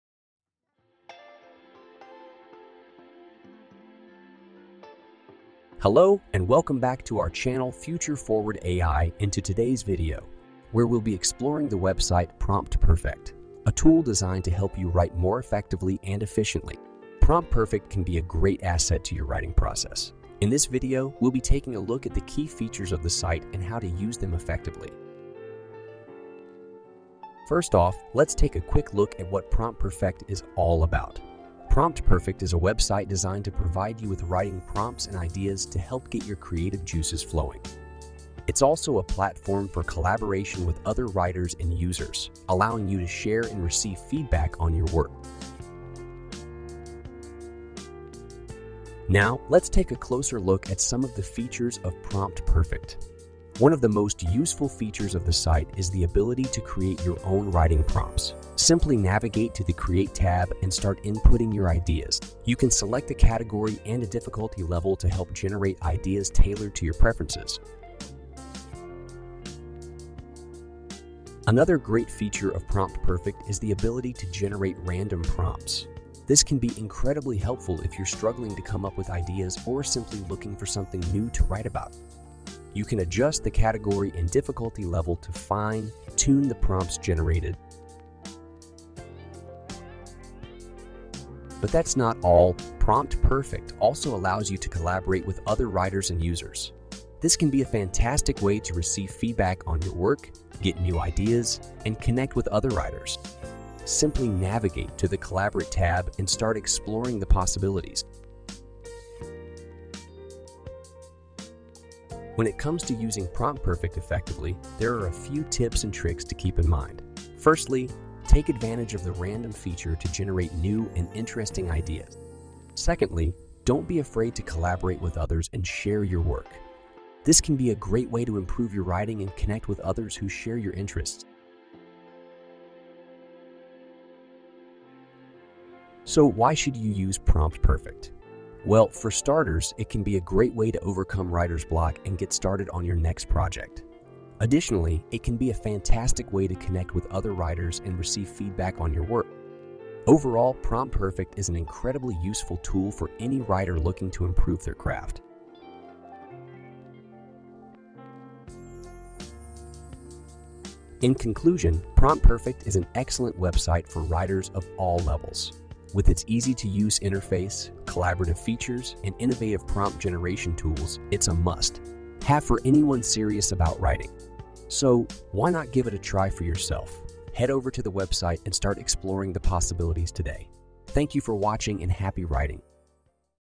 How to Use PromptPerfect for Better results on ChatGPT / Bing / Bard / Midjourney
We'll be exploring the website PromptPerfect, a tool designed to help you write more effectively and efficiently. Whether you're a seasoned writer or just starting, PromptPerfect can be a great asset to your writing process. In this video, we'll be taking a look at the key features of the site and how to use them effectively.